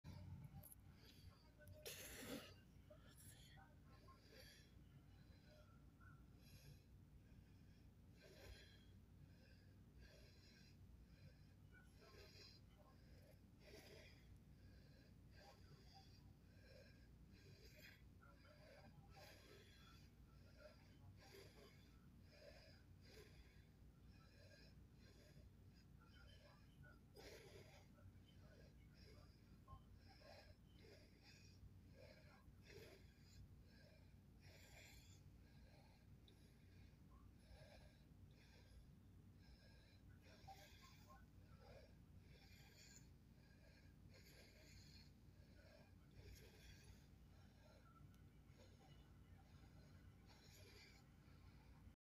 Hug & Luv Teddy Bear 2022: Kevin
Hug & Luv Teddy Bear 2022: Kevin. Momma bought it from Food Lion last year.